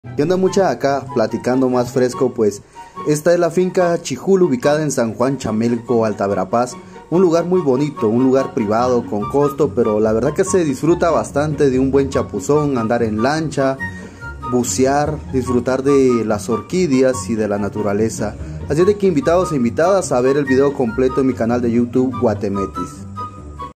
Chijul, San Juan Chamelco, Alta Verapaz 🇬🇹🥳💚🌲🌸🌼
Un lugar privado con costo, pero con muchas opciones para divertirse snorkel, SUP, senderismo, pesca, natación y mucho más. Disfrutando del clima y ambiente de Alta Verapaz.